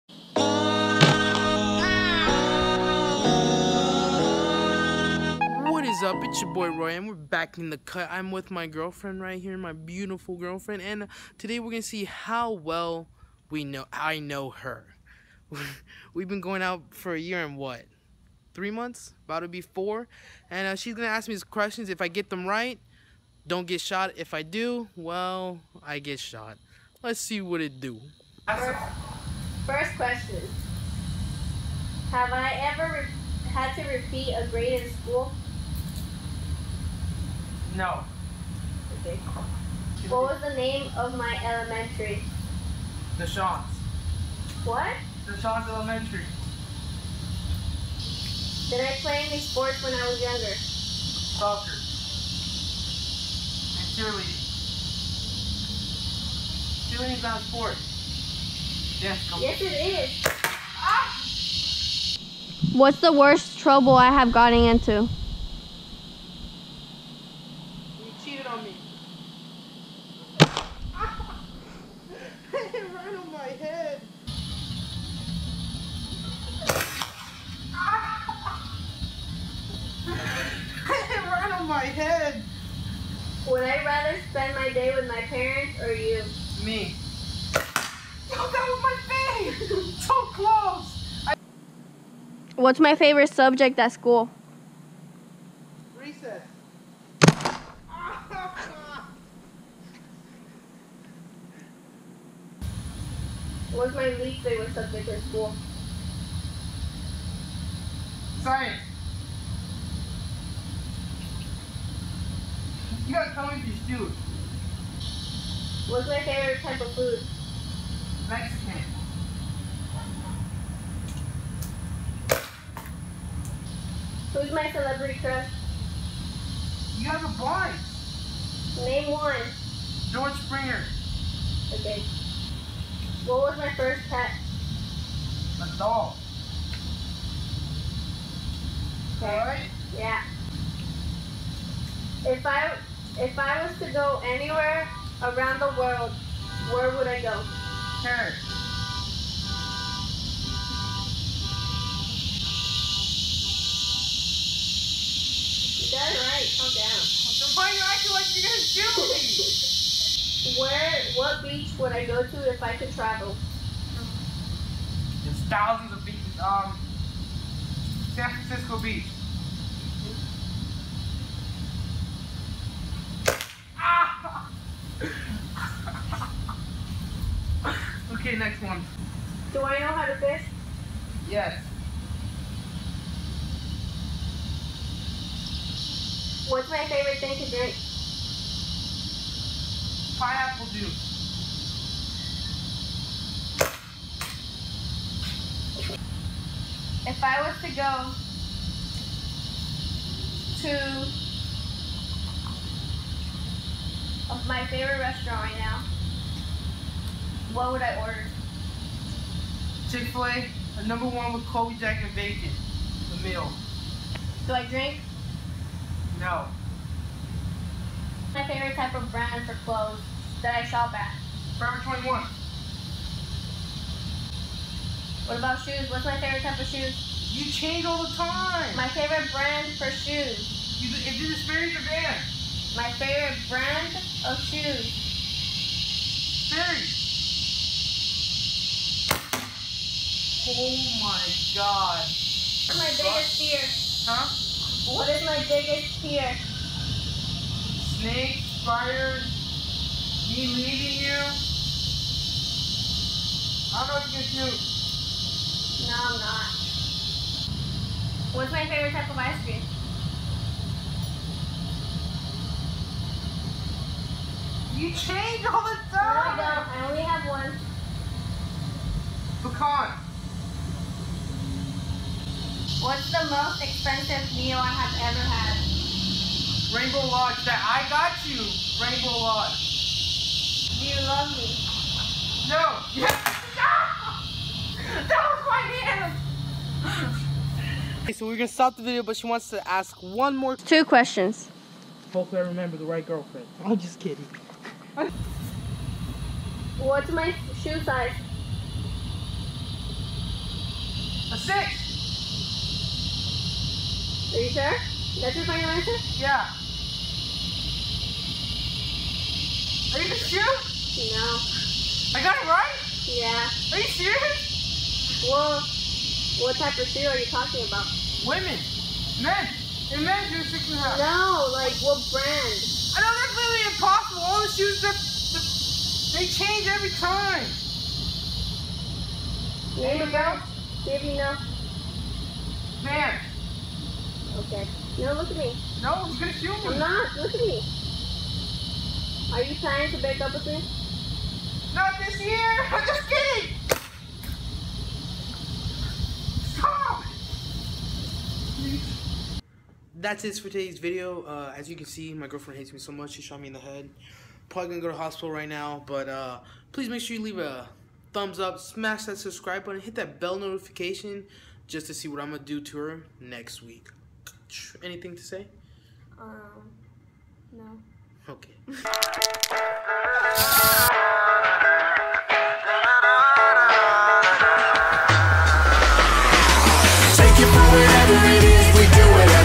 I gave my girlfriend a PAINTBALL GUN (BAD IDEA)
I'm back with yet another dope video and today I'm gonna see how well I know my girlfriend